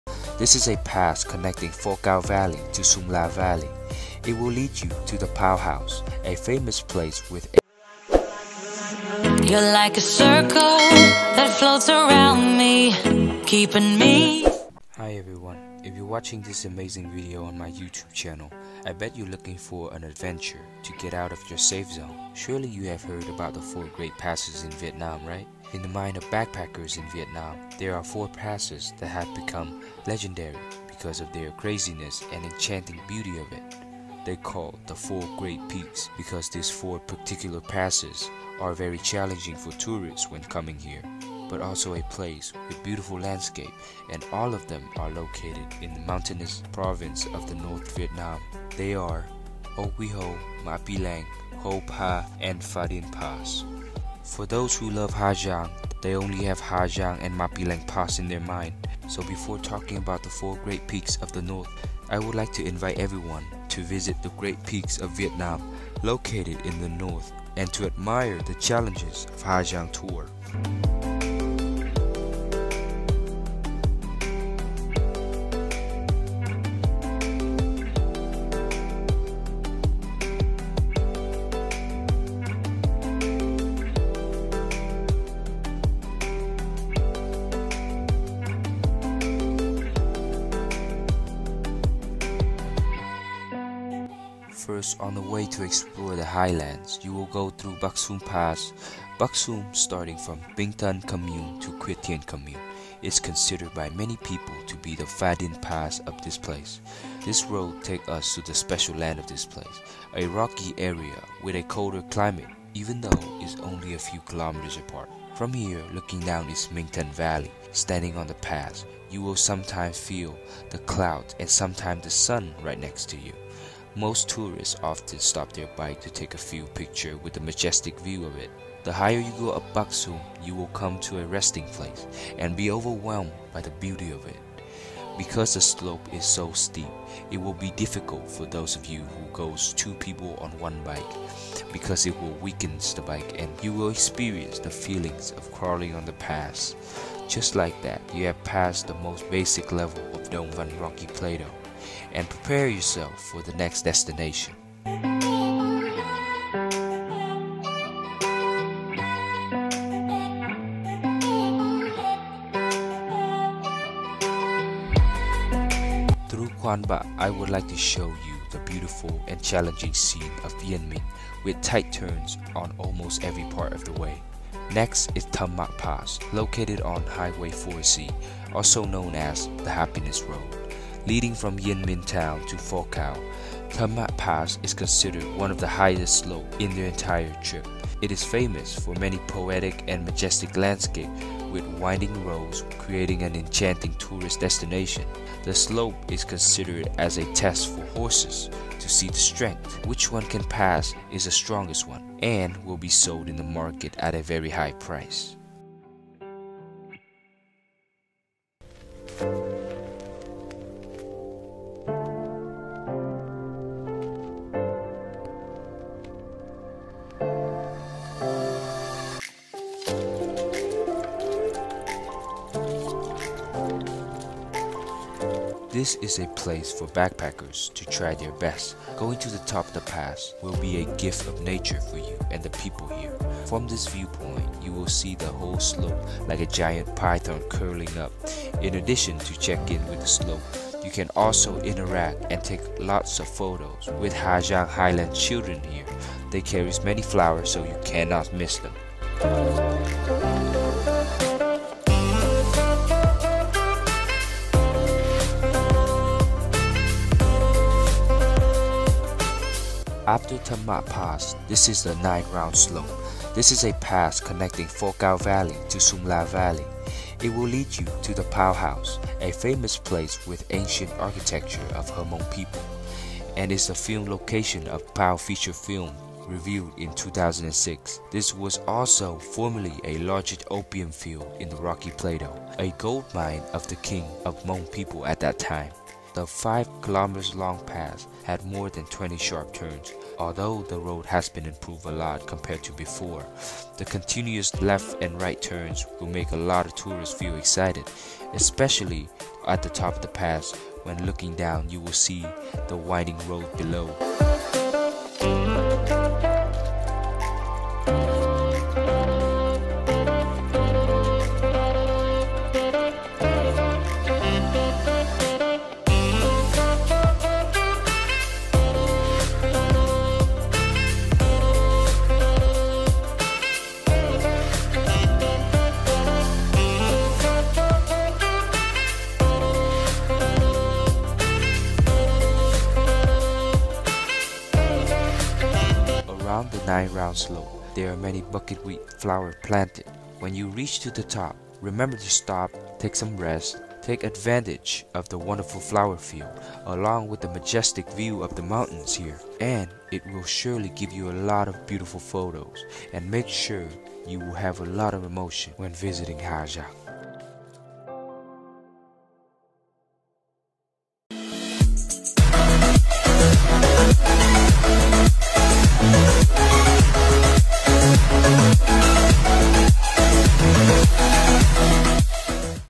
The Four Great Pass on Ha Giang Loop Tour l Jasmine Tour
1.1 Hi everyone, If you're watching these amazing videos on my youtube channel, I bet you're looking for an adventure to get out of your safe zone.

1.2 Surely you have heard about the four great passes? In the minds of backpackers, in Vietnam, there are 4 passes that have become legendary because of their craziness and enchanting beauty. They called it the four great peaks because these four particularly passes are very challenging for tourists, but also a place with beautiful landscapes, and all of them are located in the mountainous provinces of North Vietnam. These are O Quy Ho, Ma Pi Leng, Khau Pha, and Pha Din pass.

1.3 For those who love Ha Giang, they only have Ha Giang and Ma Pi Leng in mind, so before talking about the 4 great peaks of the North, I would like to invite everyone to visit the great peaks of Vietnam located in the North and to admire the challenges of Ha Giang's tour

2. First on the way to explore the highlands, you will go through Bac Sum Pass. Bac Sum starting from Minh Tan commune to Quyet Tien commune is considered by many people to be the Pha Din pass of this place. The road takes us to a special land of this place, a rocky area with a colder climate even though only a few kilometers apart. From here, looking down is Minh Tan valley, standing on the pass, you will sometimes feel like in the clouds, sometimes in the sun. Most tourists often stop their bikes to take pictures with the majestic view of the slope. The higher you go up the Bac Sum you will come to a resting